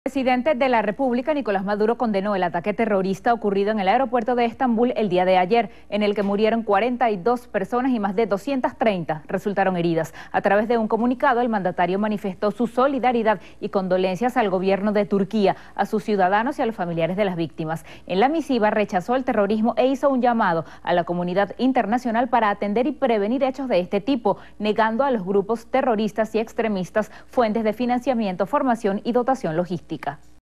Presidente Maduro condenó el ataque terrorista ocurrido en el aeropuerto de Estambul
El presidente de la República Nicolás Maduro condenó el ataque terrorista ocurrido en el aeropuerto de Estambul el día de ayer, en el que murieron 42 personas y más de 230 resultaron heridas.